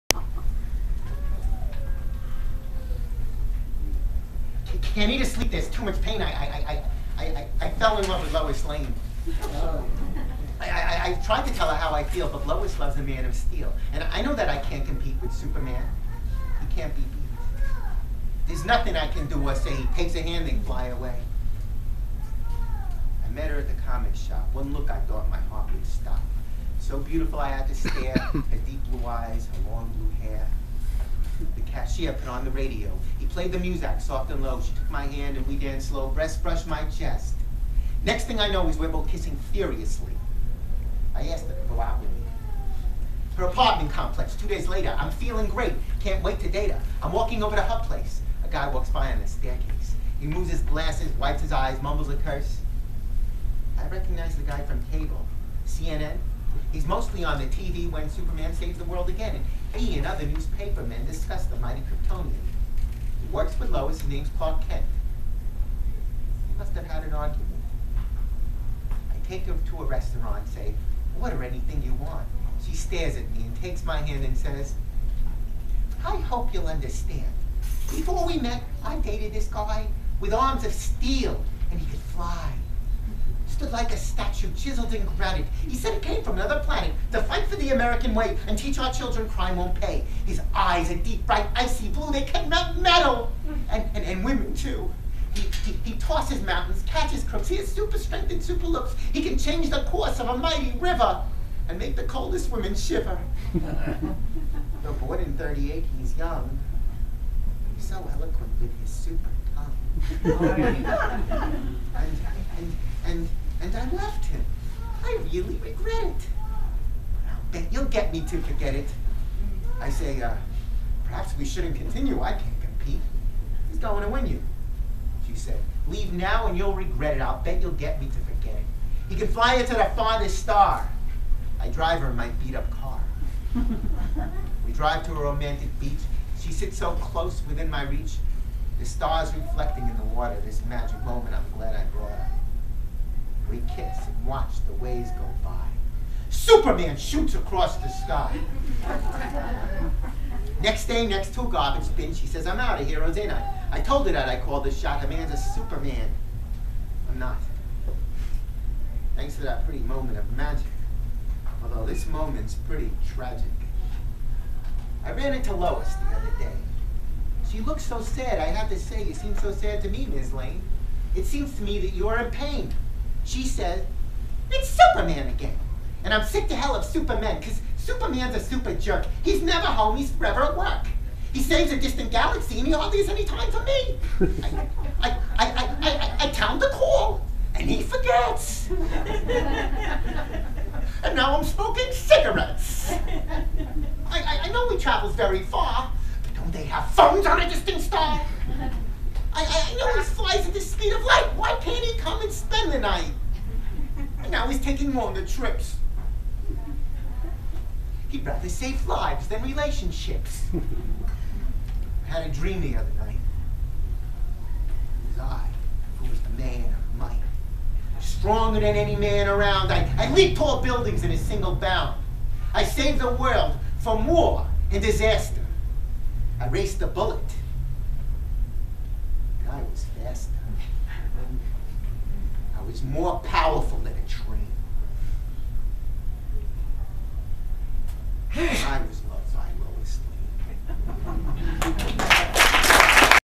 Joe Camhi - "A Comic Tragedy" - a story poem.
Encino Barnes and Noble feature reading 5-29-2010